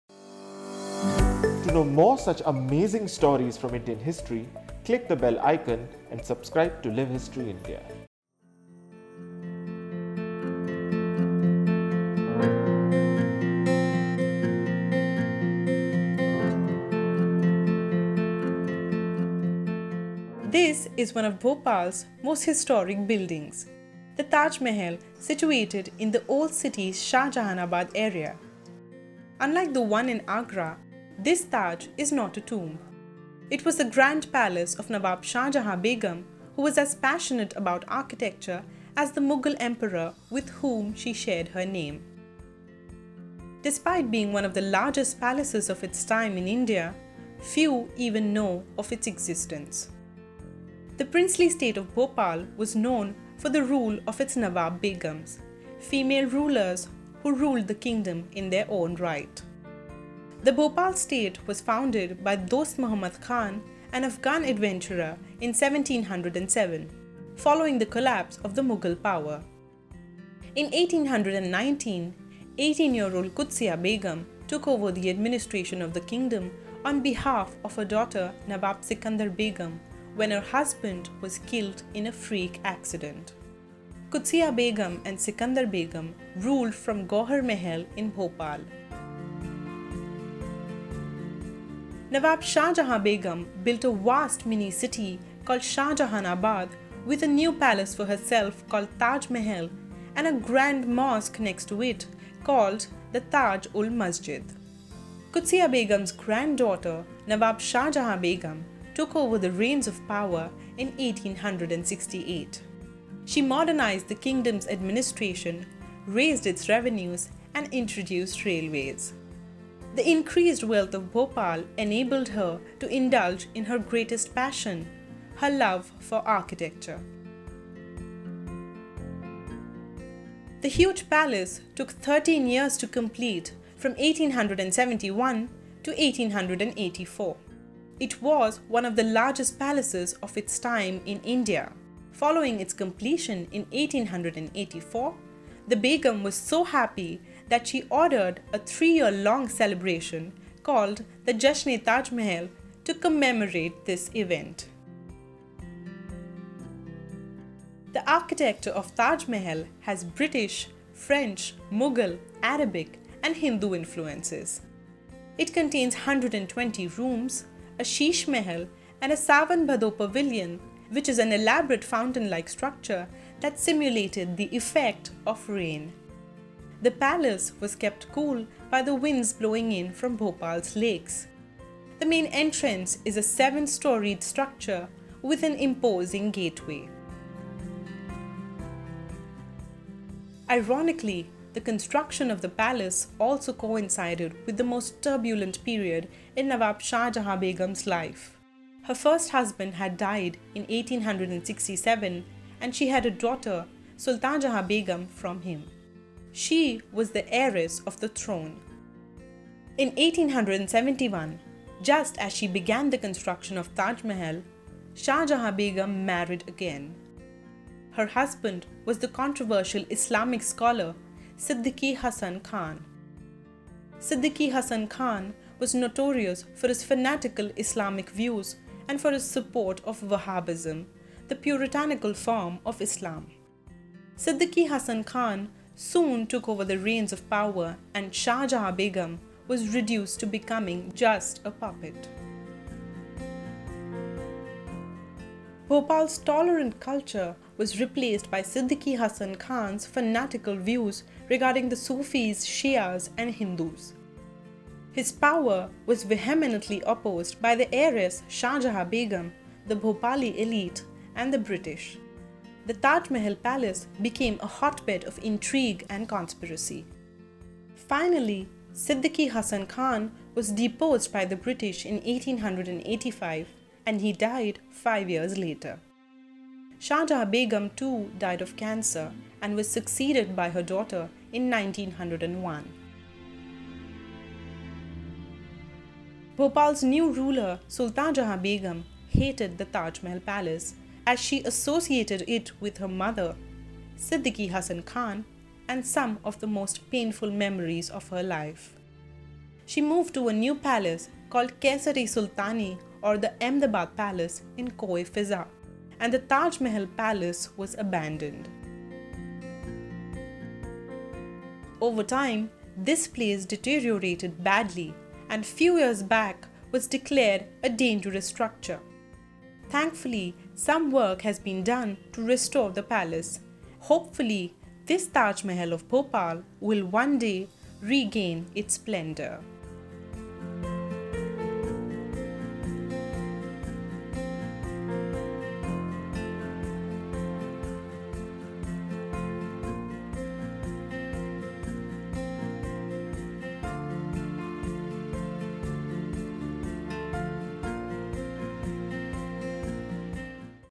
The Taj Mahal Palace of Bhopal
This is one of Bhopal’s most historic buildings - The ‘Taj Mahal’ situated in the old city’s Shahjahanabad area. Unlike the one in Agra, this Taj is not a tomb. It was the grand palace of Nawab Shah Jahan Begum who was as passionate about architecture as the Mughal emperor with whom she shared her name. Despite being one of the largest palaces of its time in India, few even know of its existence.